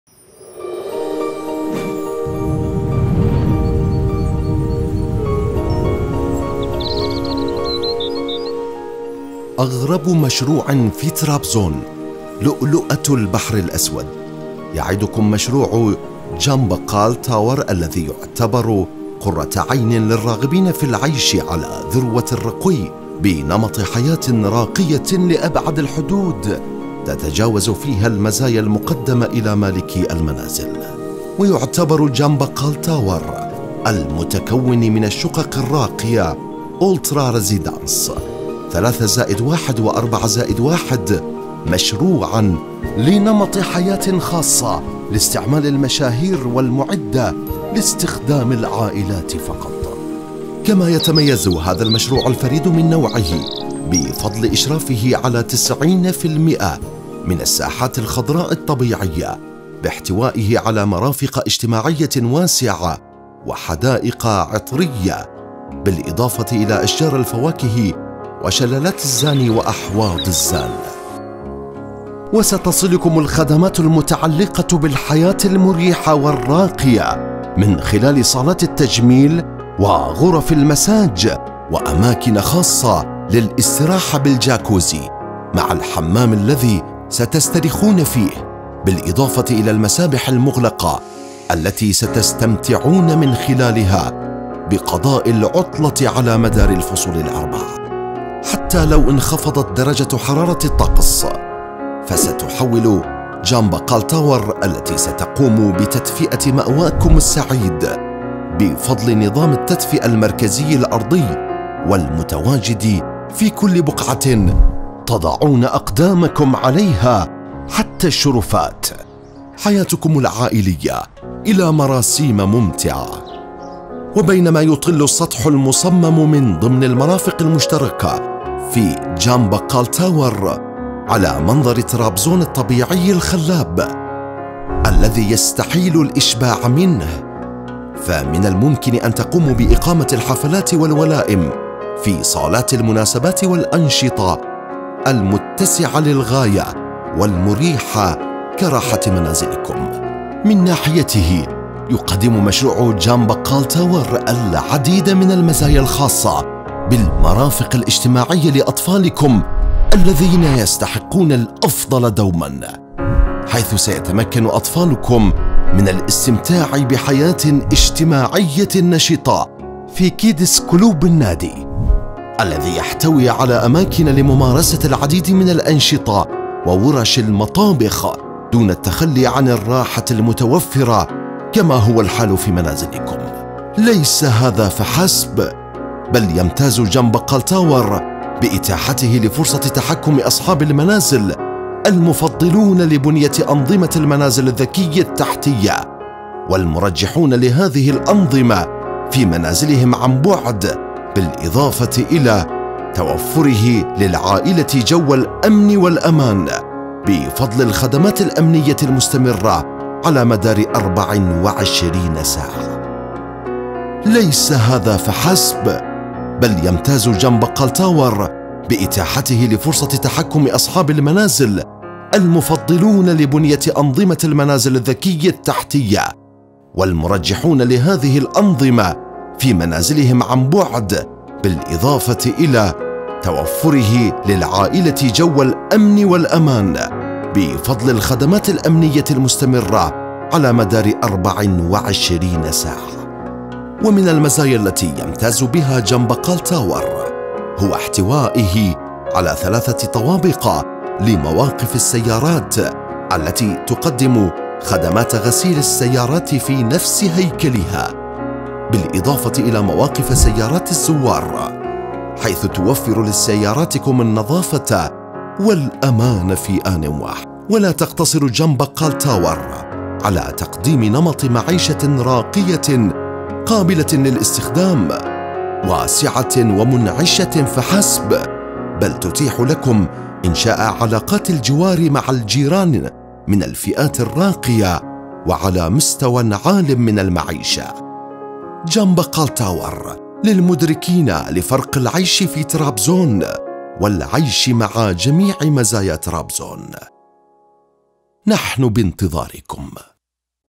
شاهد افخم مشروع في طرابزون بنظام البيوت الذكية
خذوا موقعكم في القمة

نقدم لكم اليوم مشروع يعتبر من افخم مشاريع البحر الاسود في الشمال التركي
حيث يعتبر المشروع لؤلؤة مدينة طرابزون
وعلامة فارقة في عالم الانشاءات والتطوير العقاري للمنطقة
نقدم لكم مشروع جان بقال تاور
المشروع الذي بني على مساحة 11 الف متر مربع
وهو عبارة عن برج واحد مساحته 1500 متر مربع
وفيه مساحات خضراء وجلسات خارجية تقدر بـ 9500 متر مربع

يتكون المشروع من شقق بنوعين وهي /
شقق  4 غرف وصالة مساحتها 295 متر مربع ( بها تراس وبلكونة مساحتها 40 متر مربع )
شقق 3 غرف وصالة مساحتها 260 متر مربع

ـ اجمع ما يميز المشروع هو الخدمات التالية /
استقبال خاص
مواقف سيارات مغلقة ( لكل شقة موقفين )
لوبي خاص بالمجمع
مقهى وغرفة اجتماعات
صالة تجميل للرجال
وصالة تجميل خاصة بالنساء
مركز صحي متكامل يتكون من ( جاكوزي ـ حمام تركي ـ غرفة مساج ـ غرف تجميل ـ نادي صحي ـ مسبح ـ حضانة ـ والكثير غيرها )

يبعد /
1 دقيقه عن مول جواهر
5 دقيقه عن المطار
1 كم عن مستشفى المنطقة التخصصي
5 كم عن كلية الطب
مسافة مشي على االقدام من منتجع كاش اوستو
45 دقيقه عن دير سوميال
60 دقيقه عن بحيرة ازونحول
90 دقيقه عن جورجيا
120 دقيقه عن ايدر
30 دقيقه عن مزرعة الخيول

الاسعار تبدأ من مليون و 80 الف ليرة تركية وتخلتف حسب الادوار

مشروع مميز بخدمات مميزة وموقع مميز

محمد بخاري
رابط الواتساب المباشر للتحدث معي